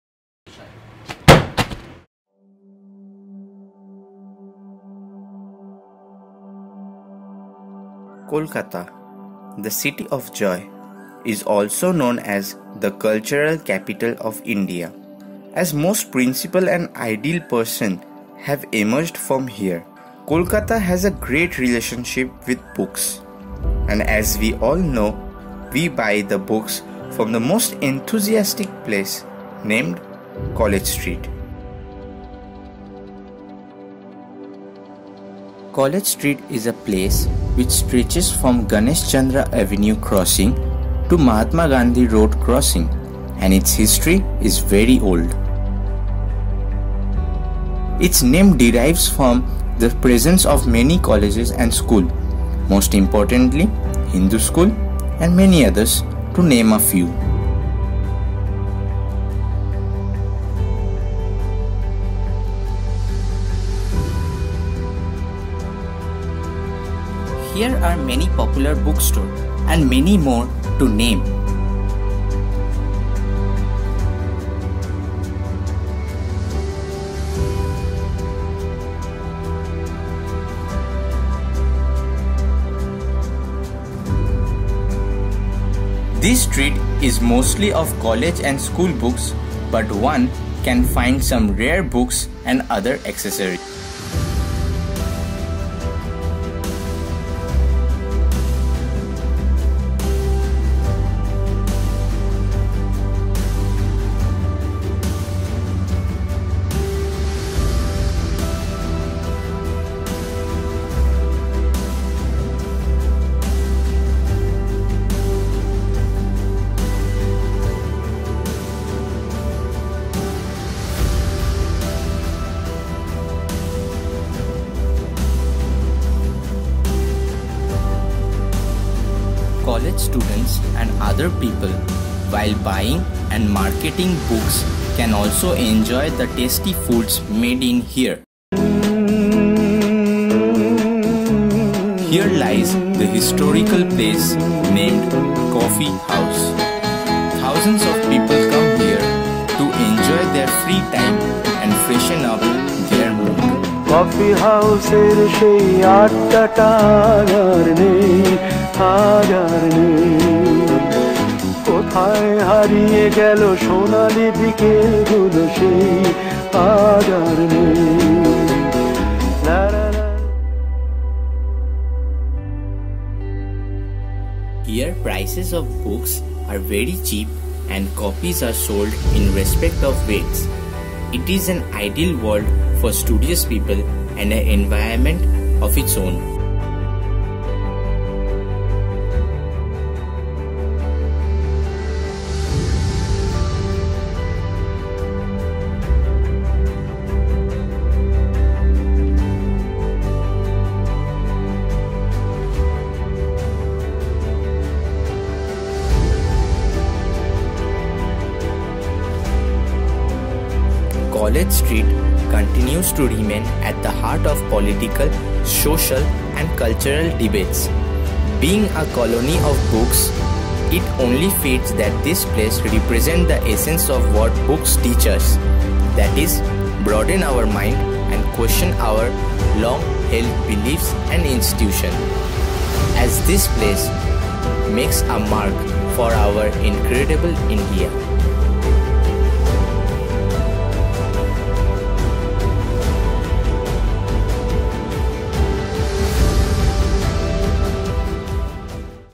College Street- A Documentary
Documentary of college street a street full of books and emotions.